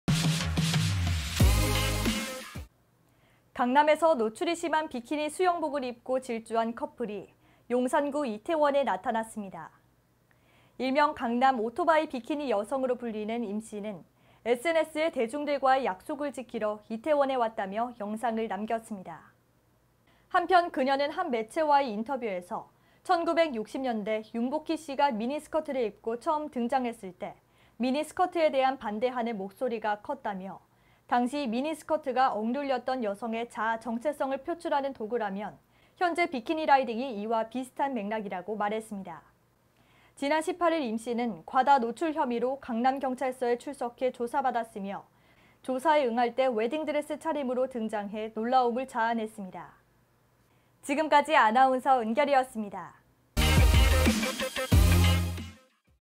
비키니 오토바이 커플 이태원에 등장해 경북신문
강남에서 노출이 심한 비키니 수영복을 입고 질주한 커플이 용산구 이태원에 나타났습니다.

일명 강남 오토바이 비키니 여성으로 불리는 임씨는 SNS에 대중들과의 약속을 지키러 이태원에 왔다며 영상을 남겼습니다.

한편 그녀는 한 매체와의 인터뷰에서 1960대 윤복희씨가 미니스커트를 입고 처음 등장했을 때 미니스커트에 대한 반대하는 목소리가 컸다며 당시 미니스커트가 억눌렸던 여성의 자아 정체성을 표출하는 도구라면 현재 비키니 라이딩이 이와 비슷한 맥락이라고 말했습니다.

지난 18일 임씨는 과다 노출 혐의로 강남경찰서에 출석해 조사받았으며 조사에 응할 때 웨딩드레스 차림으로 등장해 놀라움을 자아냈습니다.